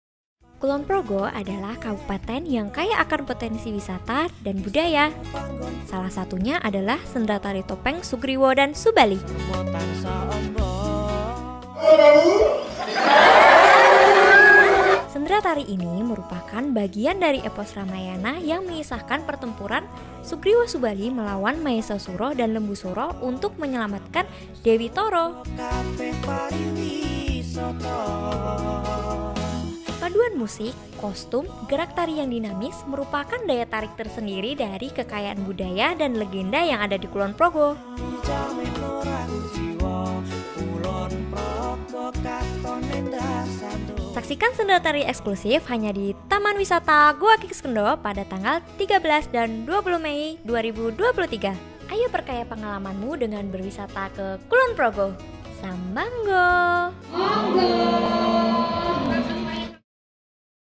Video Kreatif BBWI | Seni dan Budaya Kulon Progo Yang Mempesona
Kisah epik Sugriwa-Subali menghadirkan kehebatan pertempuran untuk melawan kejahatan dan melindungi kebenaran. Lewat sendratari ini, Kabupaten Kulon Progo mempertahankan warisan budaya yang kental dengan nilai keberanian dan persatuan dalam menjaga harmoni kehidupan. Mari bergabung dan terpesona dengan gerakan indah Sugriwa-Subali yang memikat dalam upaya menyelamatkan Dewi Toro.

dinpar.kulonprogokab.go.id dan pesona.kulonprogokab.go.id